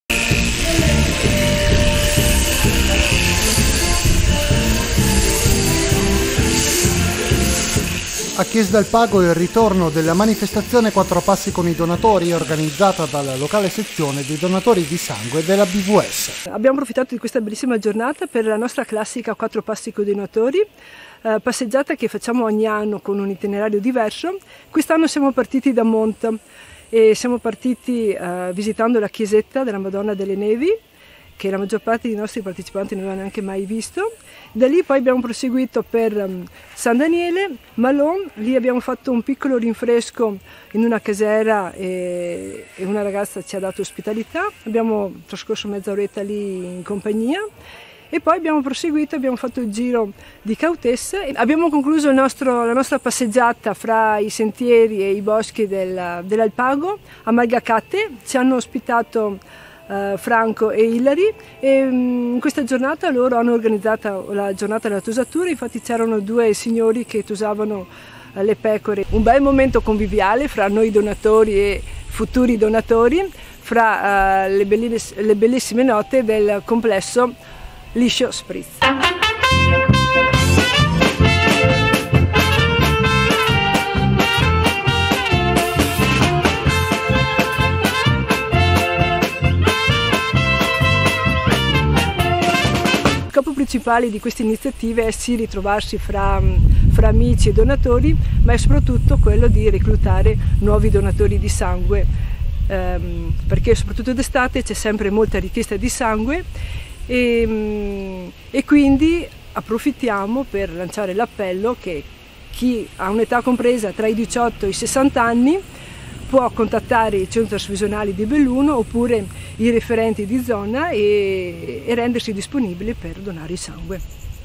A Chies d'Alpago, "Quattro passi con i donatori"
L'appuntamento, organizzato dall'ABVS, serve anche per avvicinare nuovi volontari